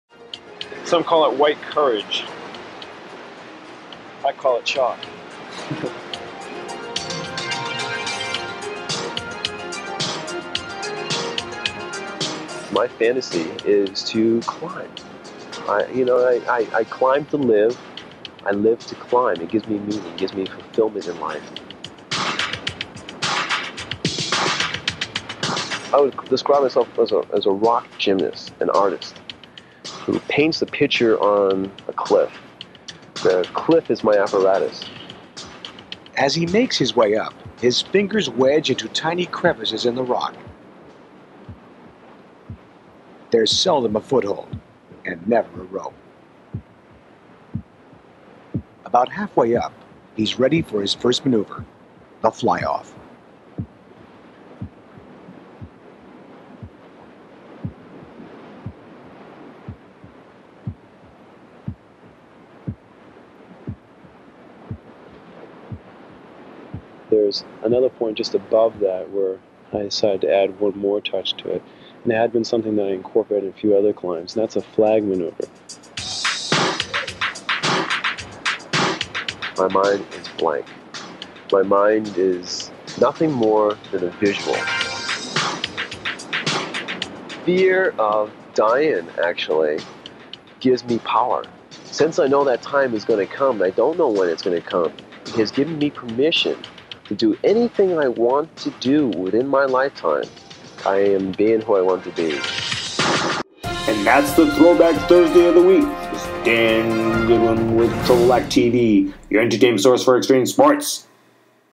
ThrowBack Thursday! Featuring Dan Goodwin ~ Founder of TripleBlack TV Soloing Mickey's Beach Crack!
THROWBACK THURSDAY!  Featuring DAN GOODWIN ~ Founder of TripleBlack TV Free Soloing Mickey's Beach Crack 5.12b for National TV!  ‪